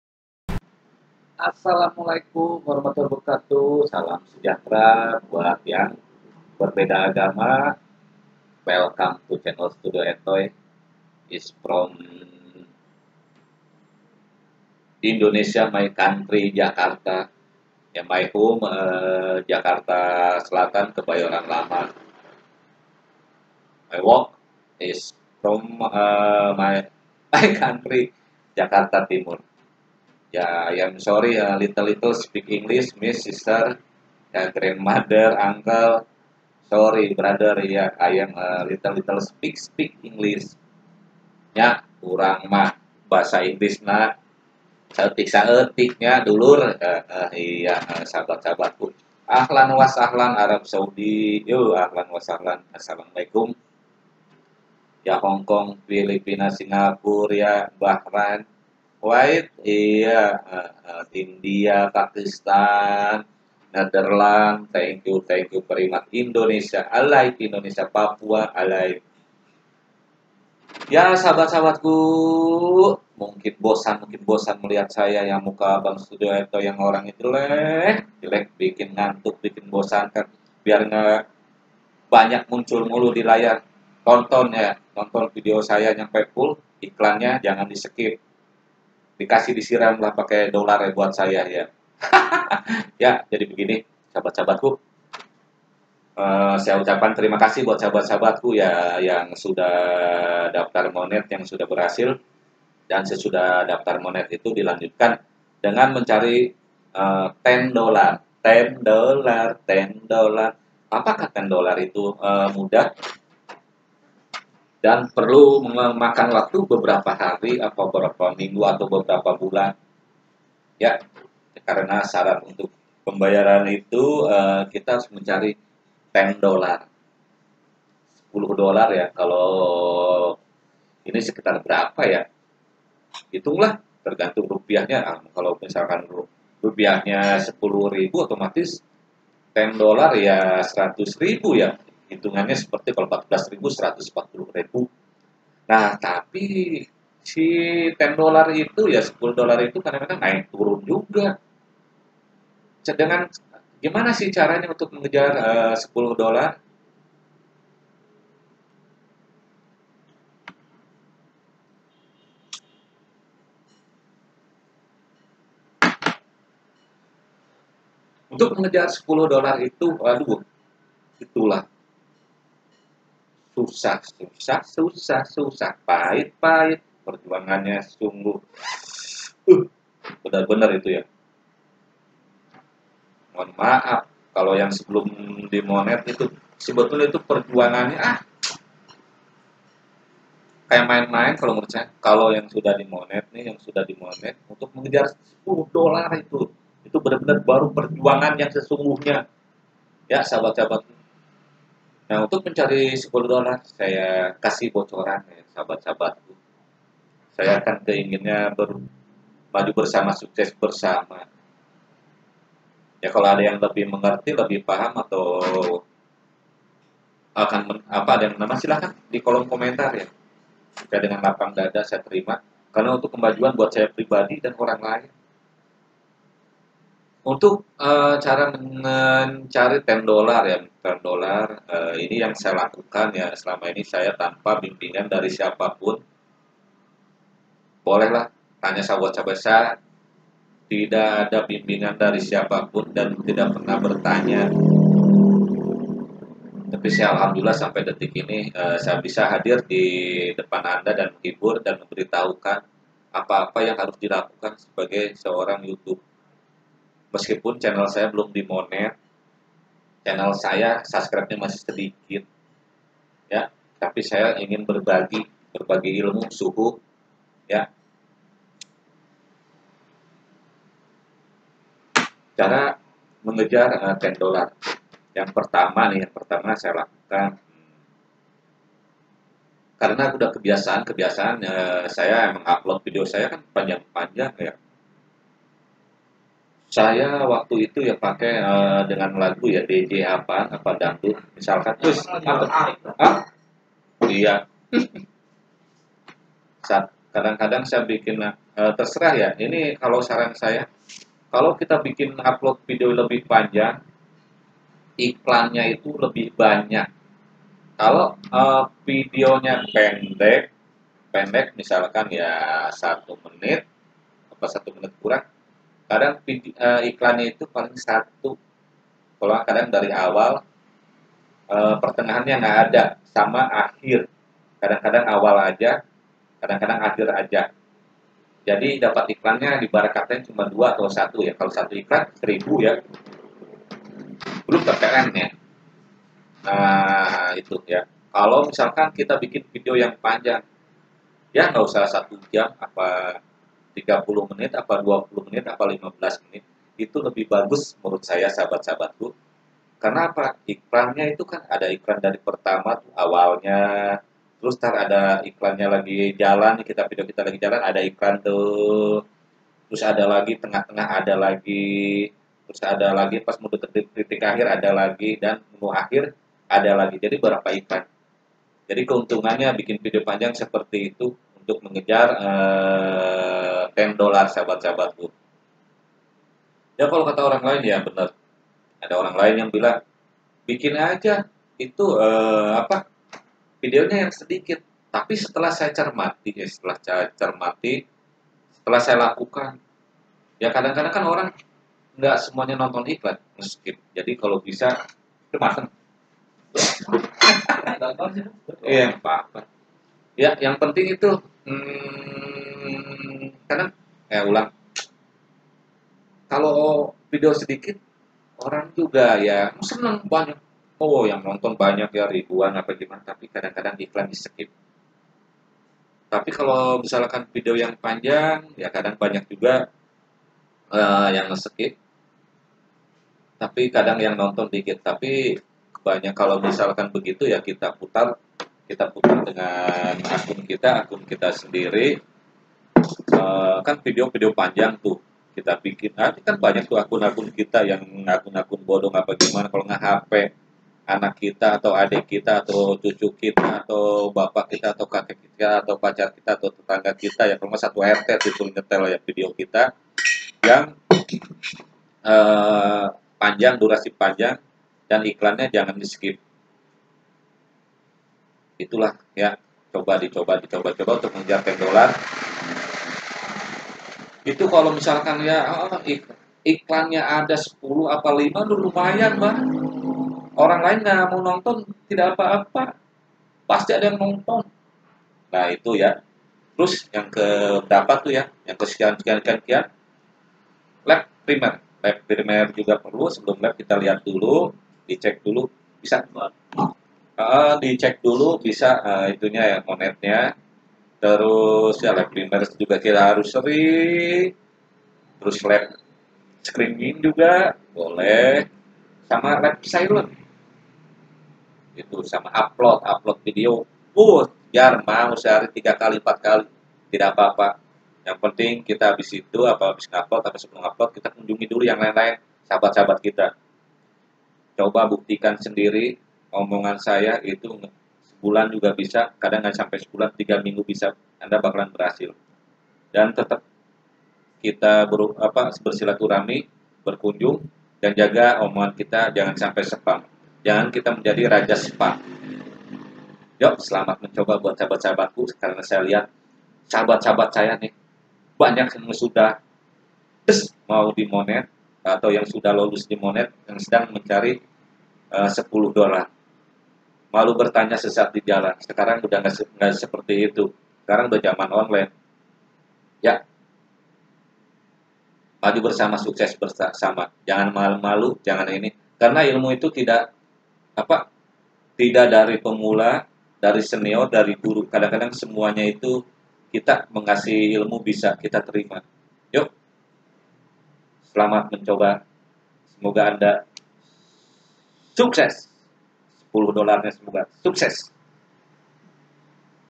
Cara Jitu Mencari 10$ Ampun Suhu... kita buka rahasia Youtuber senior
Cara Jitu Mencari 10$ Kita Ampun Suhu kita buka rahasia Youtuber senior
Inilah Perjuangan Yang Sesungguhnya
Trik
Uplod Video sehari  3Kali
uplod video durasi pendek
upload video Durasi Panjang
Live Streeming sehari sekali
live Primer sehari Sekali
live Silent sehari sekali
kita puter sendiri Video yg Panjang
Dengan akun yg lain
NOSKIP

Cara mengejar 10$
cara memcari sub
cara mengejar JT
cara Upload Video
cara edit video

Salam

STUDIO ETOY